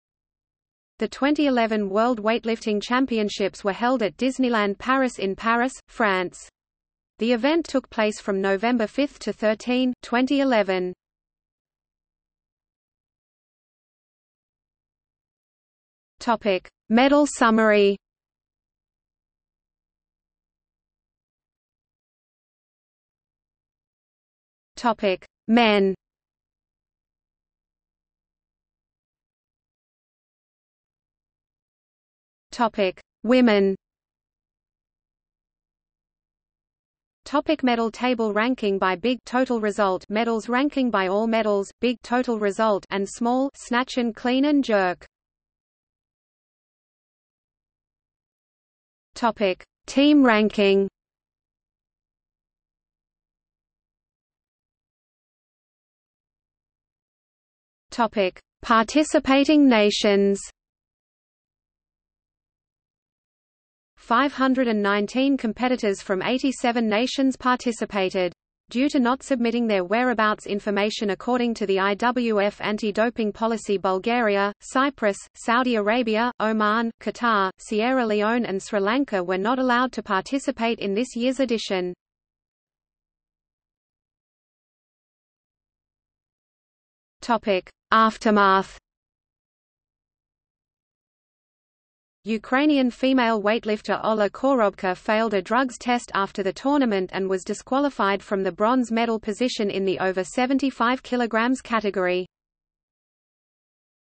2011 World Weightlifting Championships | Wikipedia audio article
00:00:12 1 Medal summary
00:00:21 1.1 Men
00:00:29 1.2 Women
00:00:37 2 Medal table
00:00:45 3 Team ranking
00:00:54 3.1 Men
00:01:23 3.2 Women

- Socrates

SUMMARY
The 2011 World Weightlifting Championships were held at Disneyland Paris in Paris, France. The event took place from November 5 to 13, 2011.